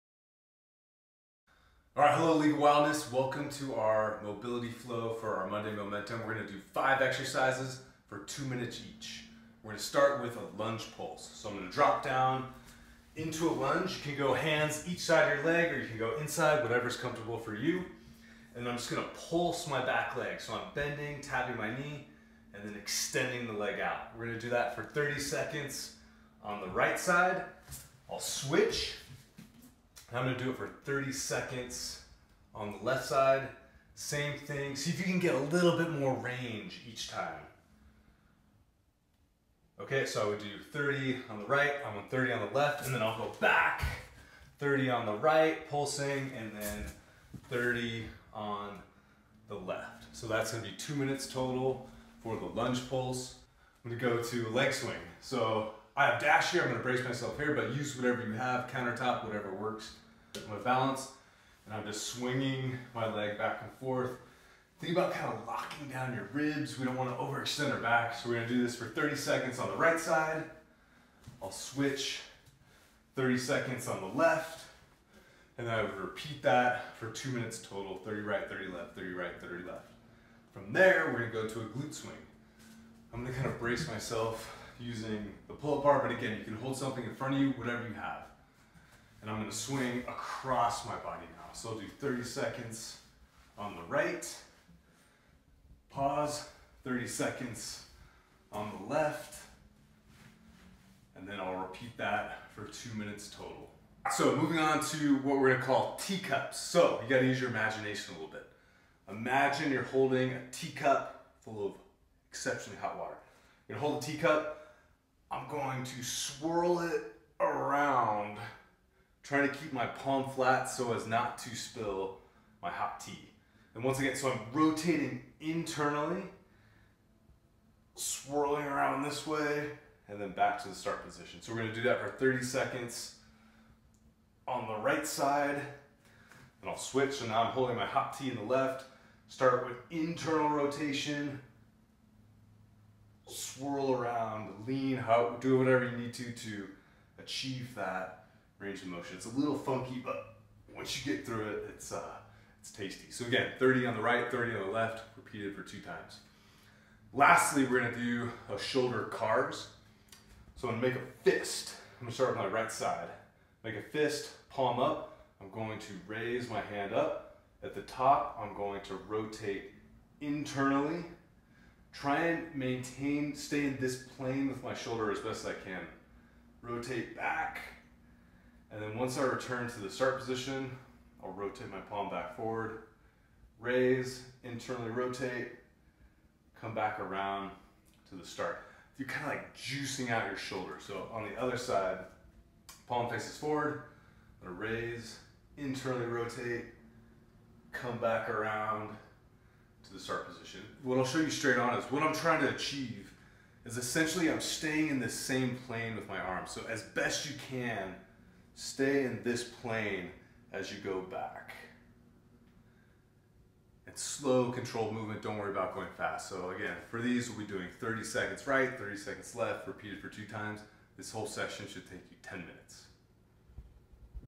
Mobility Flow 5.1.23
Mobility Flow

1. Lunge Pulse R/L Alt 30sec each leg for 2min total.
2. Leg Swing R/L Alt 30sec each leg for 2min total.
3. Glute Leg Swing R/L Alt 30sec each leg for 2min total.
4. Tea Cups R/L Alt 30sec each arm for 2min total.
5. Shoulder CARS R/L Alt 30sec each arm for 2min total.